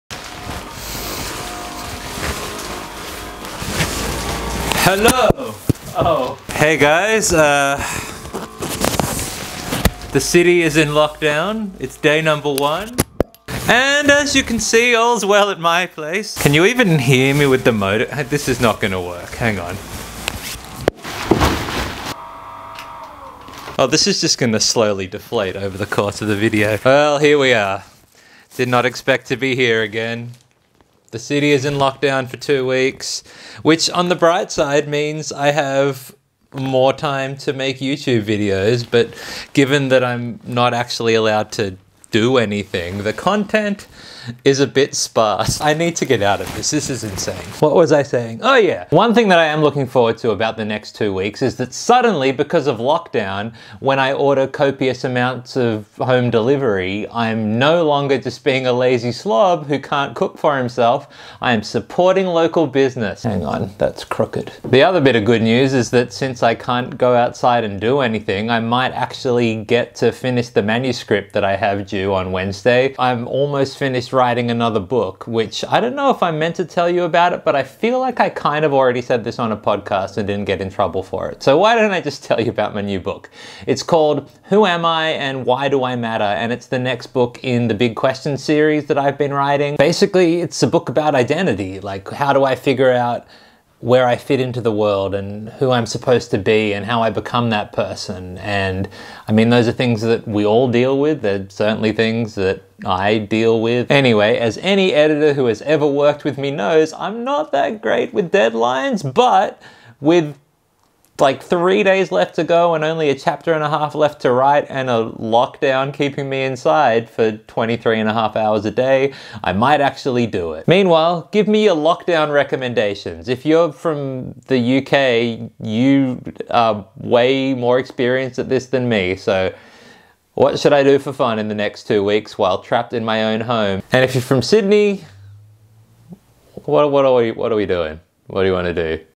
Finishing My Next Book
Leave me your lockdown activity suggestions in the comments!

Meanwhile, hopefully I'm allowed to tell you about my new book because that's what most of this video is about.

The first three books in Big Questions series are out now

HOW DO WE KNOW CHRISTIANITY IS REALLY TRUE? is a book all about the historical evidence for Jesus. Did Jesus really exist? Did Jesus really rise from the dead? (Spoiler alert: the answer is YES - but if you want more detail than that, I hope you'll check the book out!)

WHY DOES GOD LET BAD THINGS HAPPEN? is about the problem of suffering: If God is all-loving and all-powerful, why is there still so much suffering in the world?

WHAT HAPPENS WHEN WE DIE? is a book about heaven, hell, and what Jesus says eternity will be like—and, seriously, the news is way better than you think!